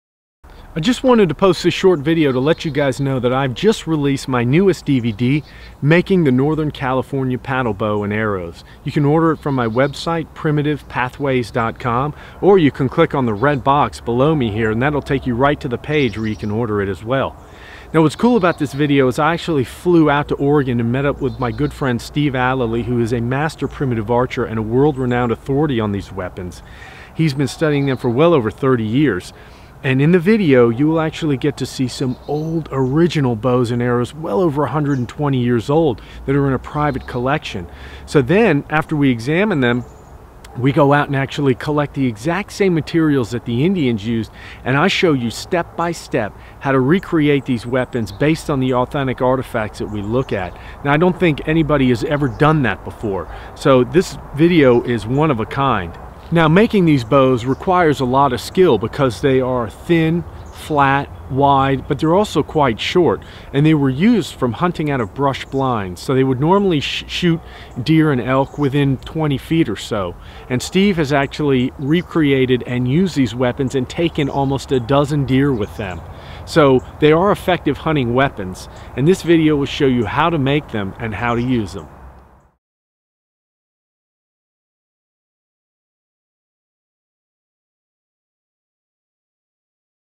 How to Make the Hupa Bow and Arrows DVD
After over 3 1/2 years of work the newest DVD has just been released!  In this video I team up with master primitive archer and artist Steve Allely.  We examine some old, original Northern CA paddle bows and arrows that are in a private collection.  Then we enter the forest to collect the very same materials the Indians used and show you, step-by-step, how to create these weapons based on the original artifacts.  There's nothing like it anywhere!  The completed weapon will be a fully functional museum-quality replica capable of taking down big game with one shot.  Run time: 67 mins.  Cost is $20 plus shipping.  International Orders welcome!